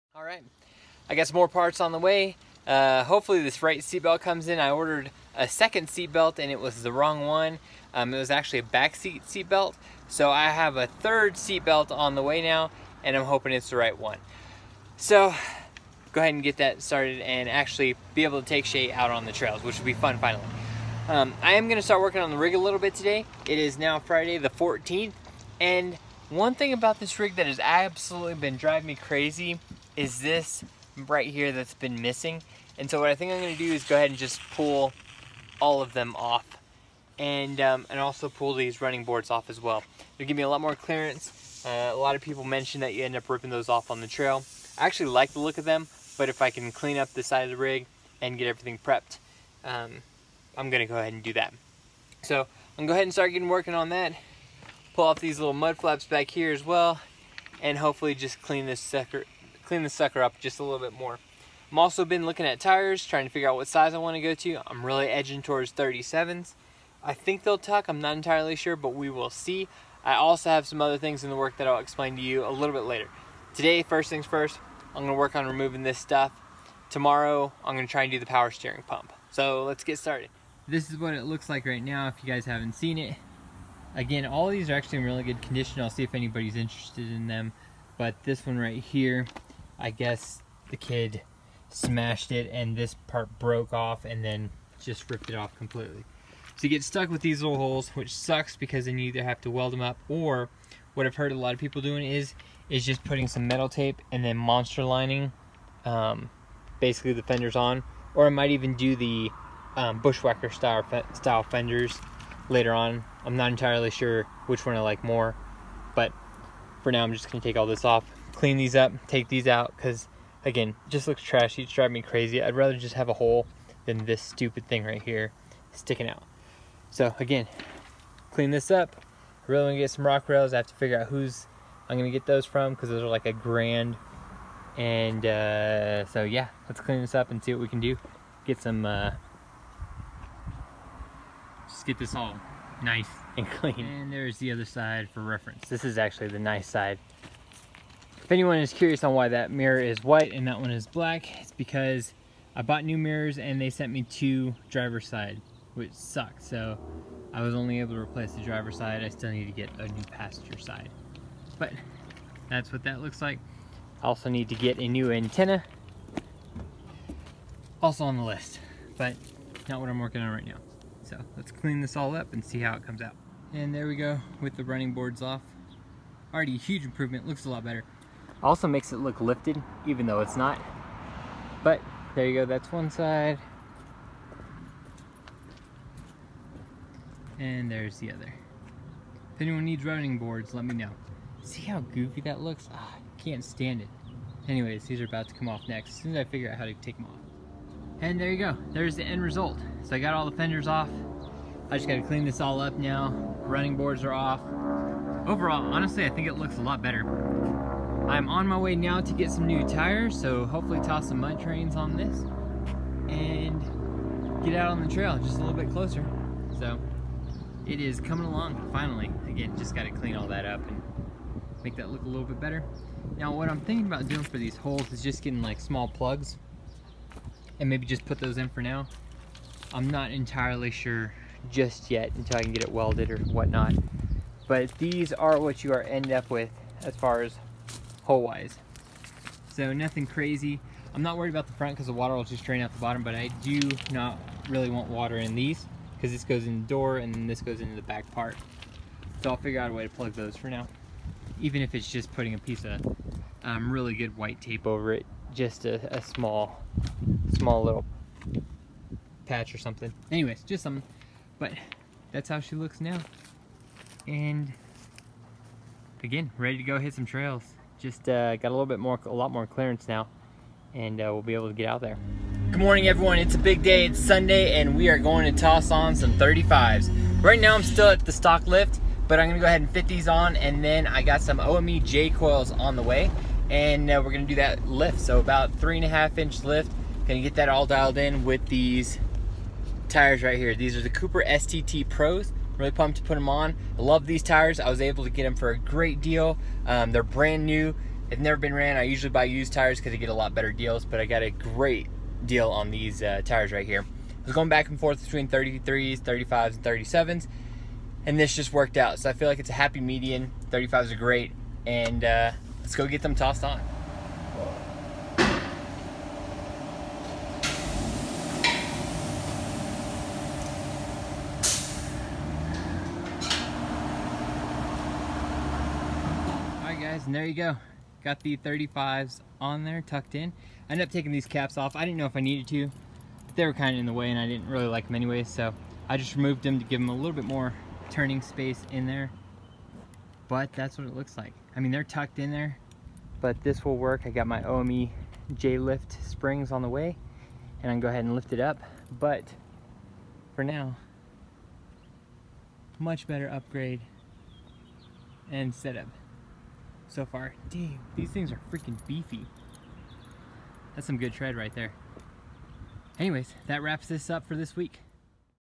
35’s on the FJ80 Land Cruiser! + Removing Fenders and Running Boards - 2019.33
Finally! Progress toward the trail rig!

Tires on! It’s a tight fit but the lift is coming shortly! For those curious, yes Shae and I are good to go, just trying to get situated! Thank you all to whom have asked!

Tekonsha Prodigy P3 Brake Controller
Valterra Sewer Hose
Surge Protector
Sewer Hose Support
Camco 25ft Drinking Hose
Camco Water Filter
Water Pressure Regulator

Tow Rig Details:

1993 Toyota FZJ80 Land Cruiser
Triple Locked
Work Done:
Full Tune Up - NGK Spark Plugs / Wires, Distributor Cap / Rotor, Radiator Cap,
Disc Brake Rotors / Pads Front and Rear
Rear Driveshaft U-Joints
FJ Cruiser Wheels
1.5" Wheel Spacers
Fluids Changed - Front/Rear Diffs, Transmission, Oil, Transfer Case
Replaced Front Inner and Outer Hub Bearings
Prodigy P3 Brake Controller
Sony 6.5” front door speakers
New drag link ends
35” Cooper STT Pro Tires
Removed Fenders and Running Boards

See you on the trail!
- FC